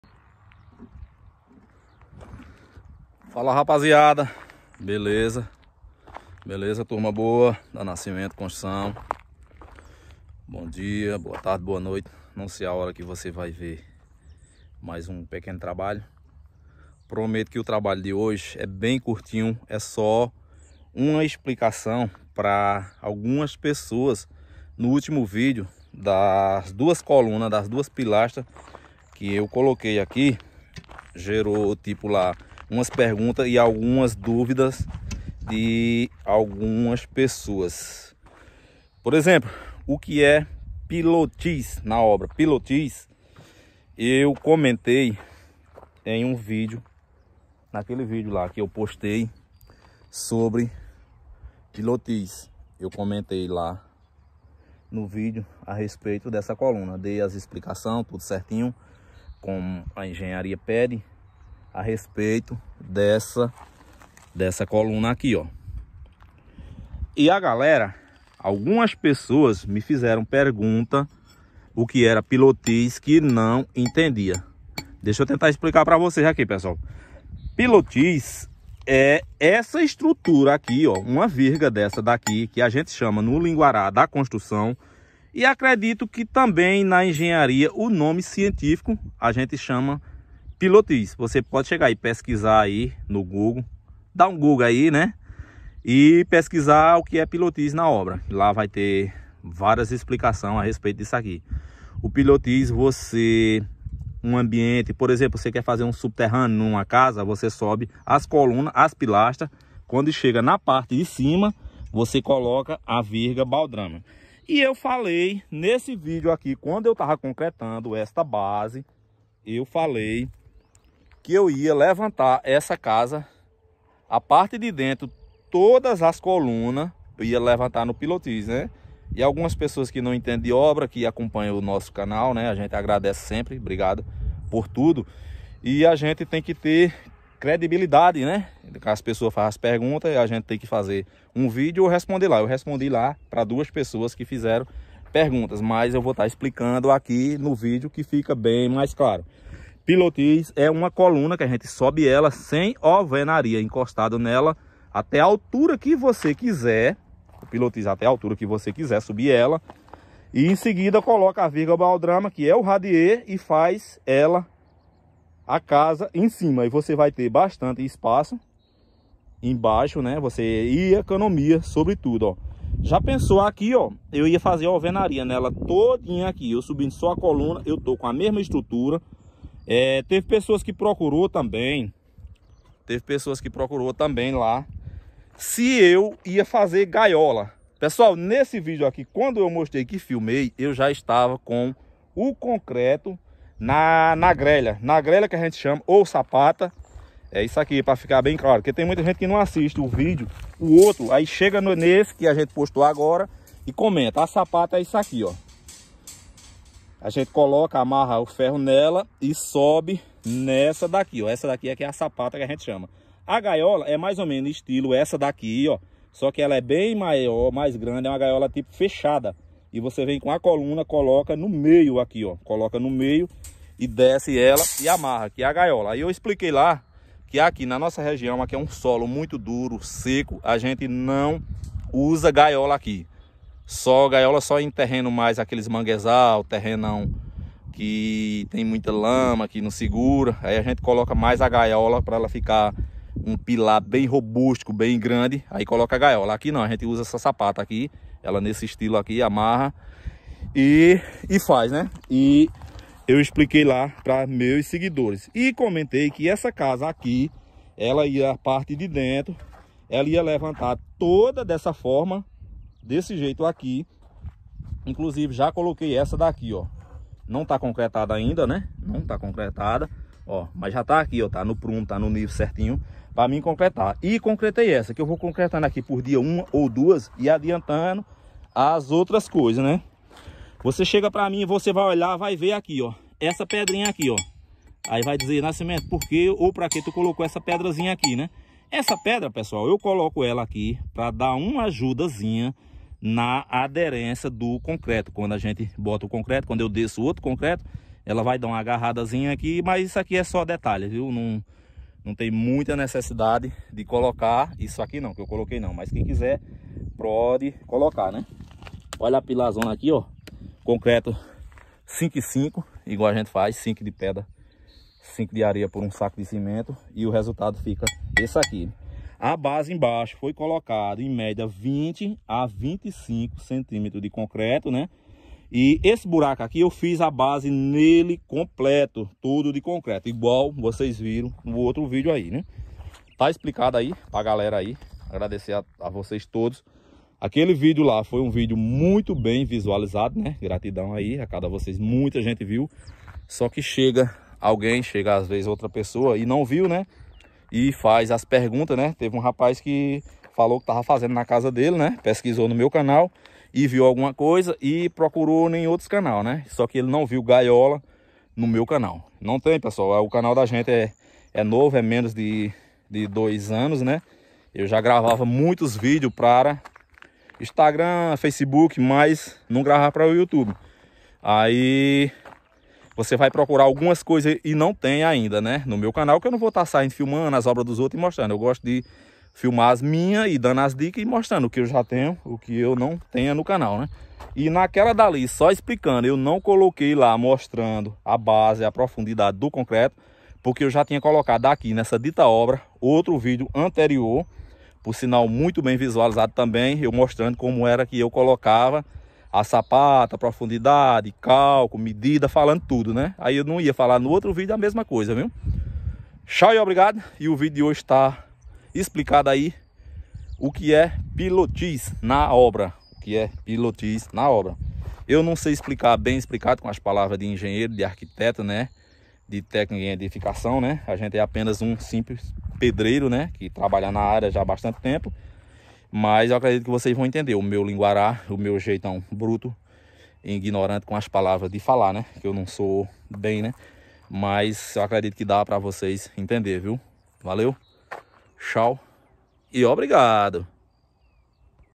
Oque e pilotis na obra ??
Oque e pilotis na obra 🚧 é uma estrutura de concreto sem alvenaria 🧱